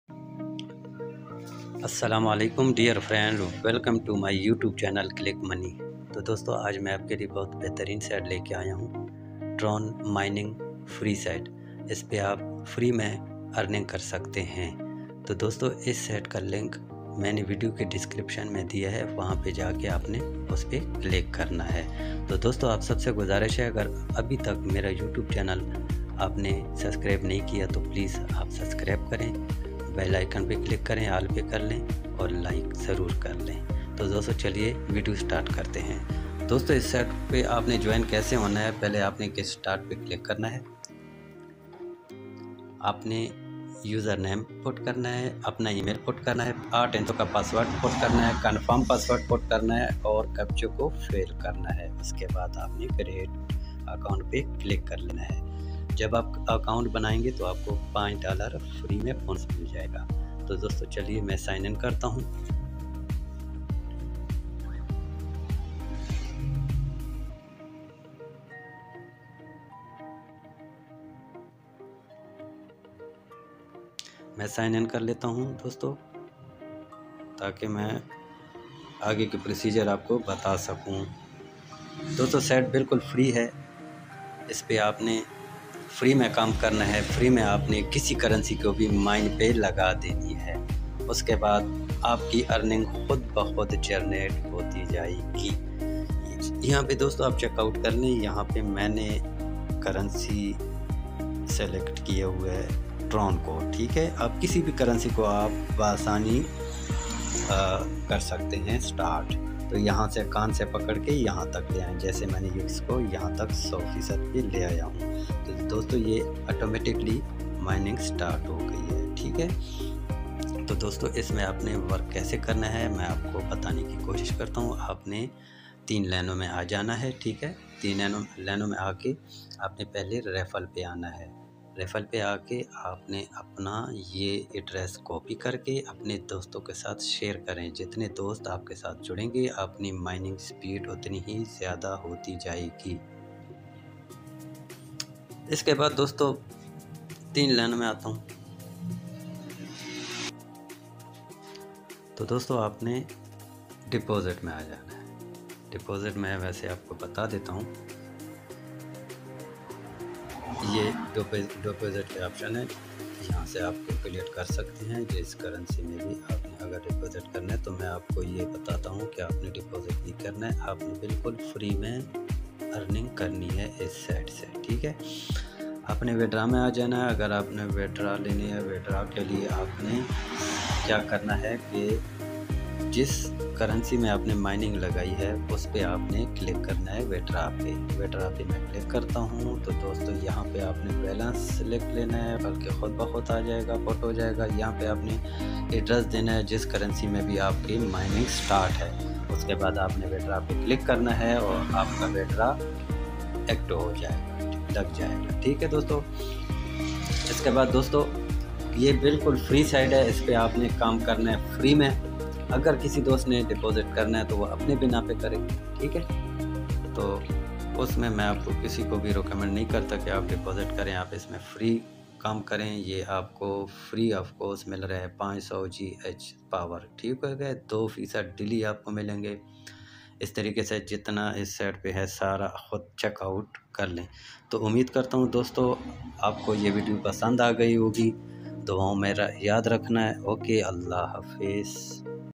Cryptocurrency Cloud Mining website l $5 sign up Bonus l Best Crypto Mining Site 2023 💰💰👍
Friends, in this video you will see about TRX Cloud Mining, how you will earn free on this site, how you will create an account and how you will withdraw from this site for free, you can see all this in this video.